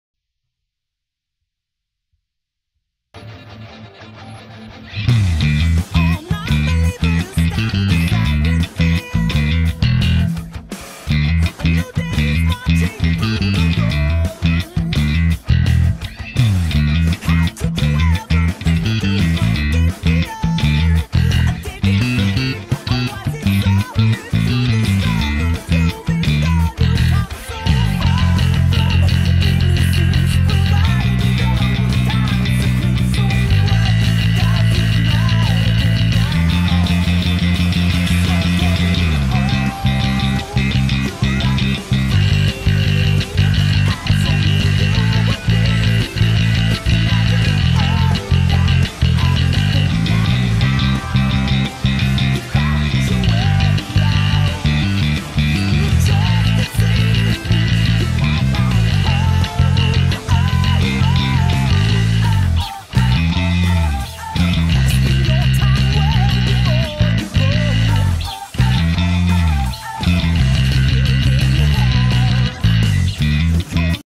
1974 Fender Jazz Bass Tone Test
Just a quick demo of the newest addition to the stable!